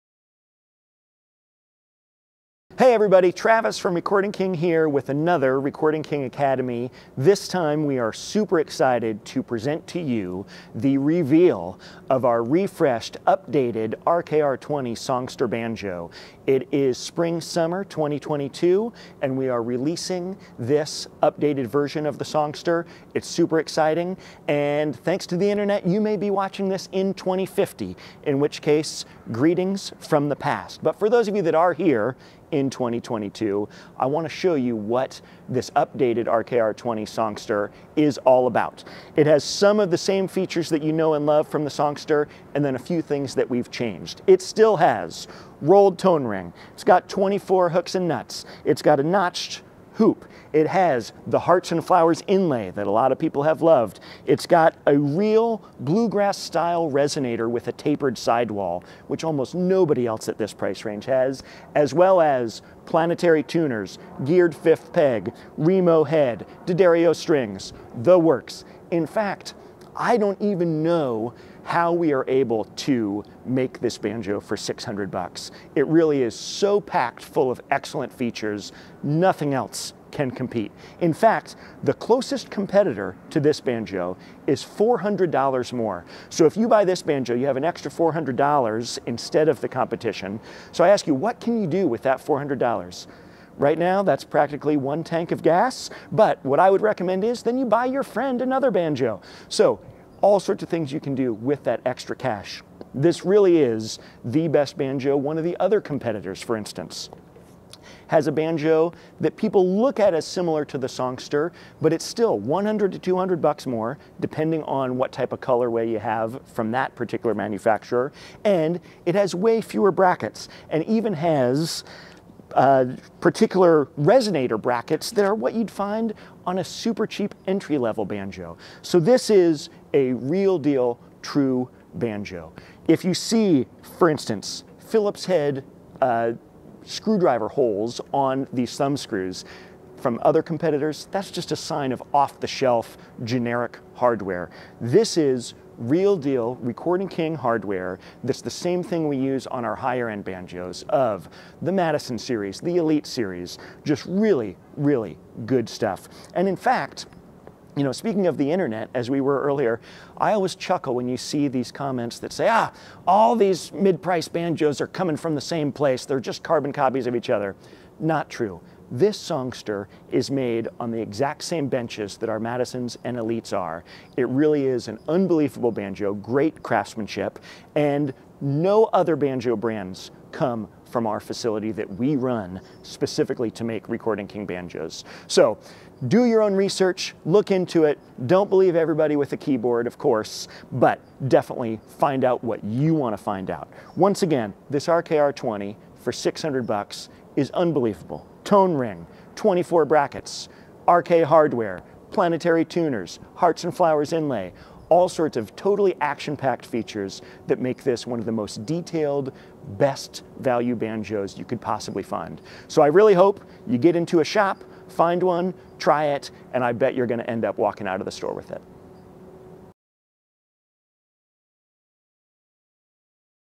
The All-New RK-R20 Songster Banjo (2022)
For over a decade, the RK-R20 Songster Banjo has been a best-selling staple in the Recording King lineup. Over the past two years, RK had the chance to give it a top-to-bottom makeover, for an updated banjo full of upgraded tone and traditional appointments, including a rolled tone ring, planetary tuners and classic Hearts & Flowers inlay.

RK's Director of Product Travis Atz shares exactly what makes this banjo so special.

• Mahogany Resonator & Neck
• Multi-Ply Rim
• 26-1/4” Scale Length
• Rolled Brass Tone Ring
• Mahogany Gloss Finish
• Planetary Tuners